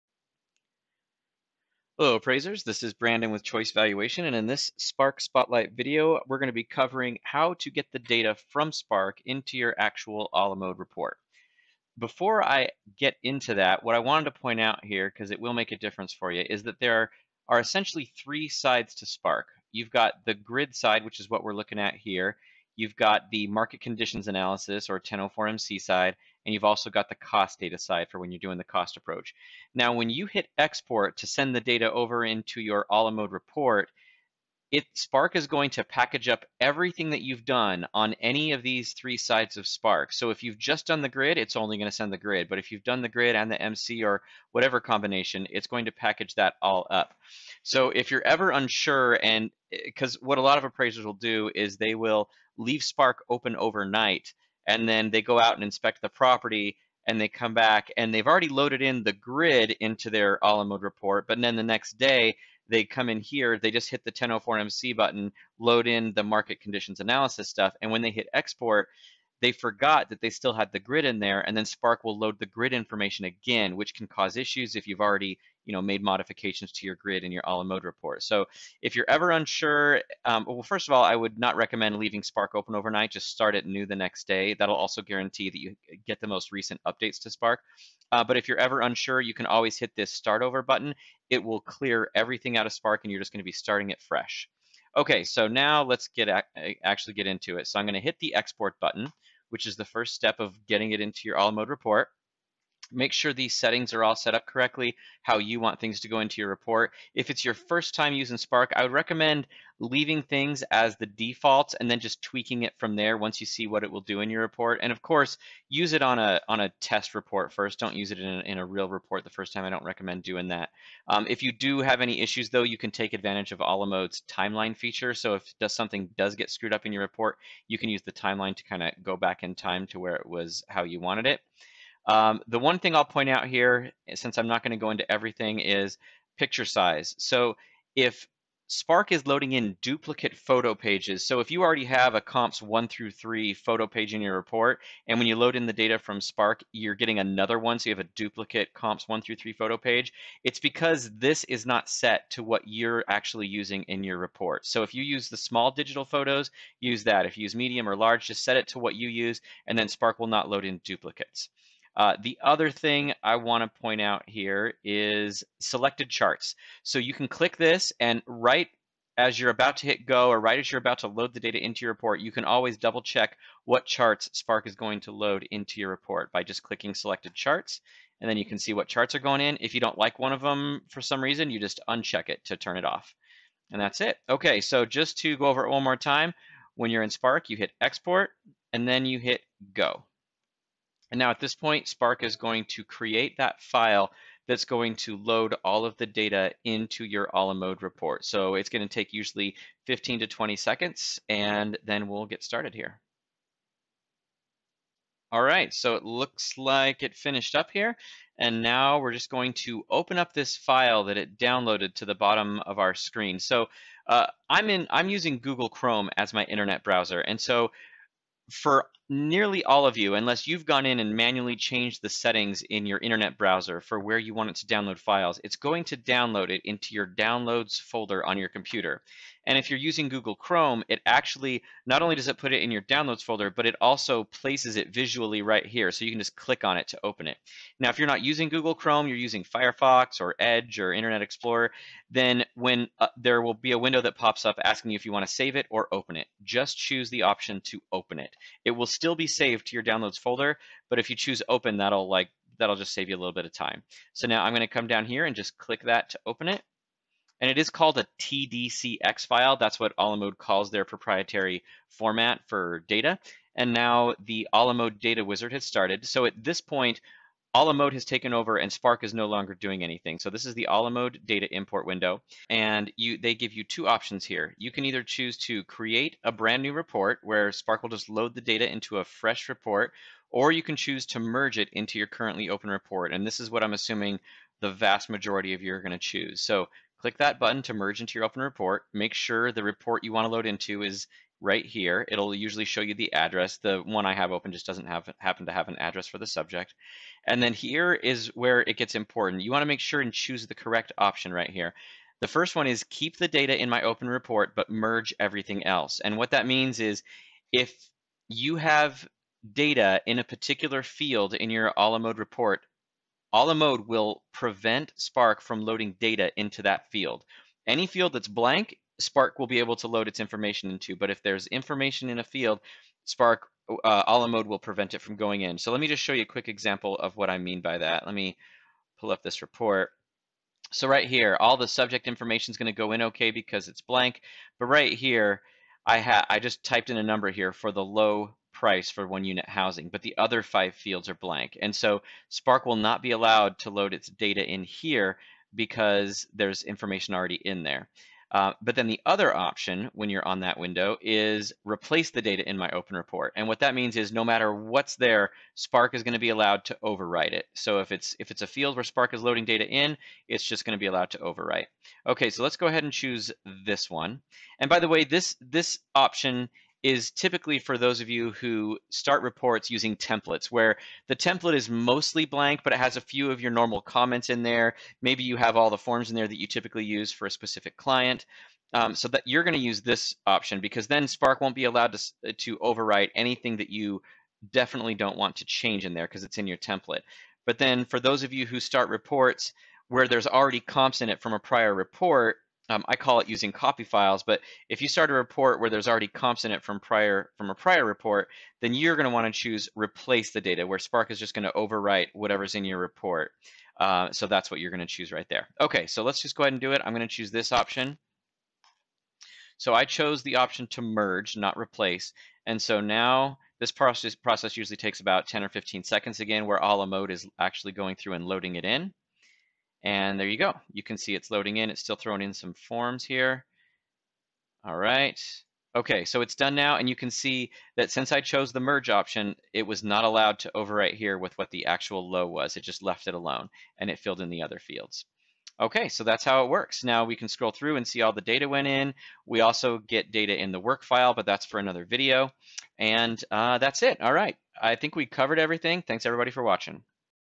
Spark Spotlight: Loading data from Spark to your TOTAL/WinTotal Report
In this video we show you how to export data to your a la mode (TOTAL or WinTotal) report and we give tips on the best ways to do it in different scenarios.